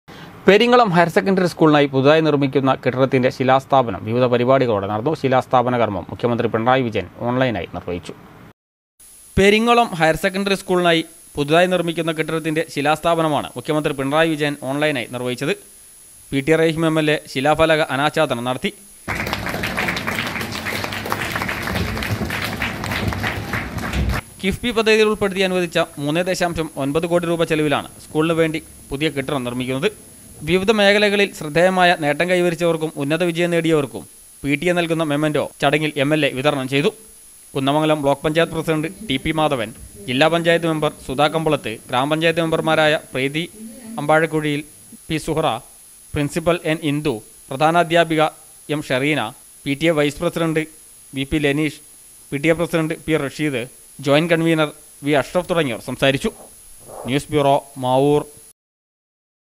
പെരിങ്ങളം ഹയർ സെക്കൻ്ററി സ്കൂളിനായി പുതുതായി നിർമ്മിക്കുന്ന കെട്ടിടത്തിൻ്റെ ശിലാസ്ഥാപനം
പെരിങ്ങളം ഹയർ സെക്കൻ്ററി സ്കൂളിനായി പുതുതായി നിർമ്മിക്കുന്ന കെട്ടിടത്തിൻ്റെ ശിലാസ്ഥാപനം വിവിധ പരിപാടികളോടെ നടന്നു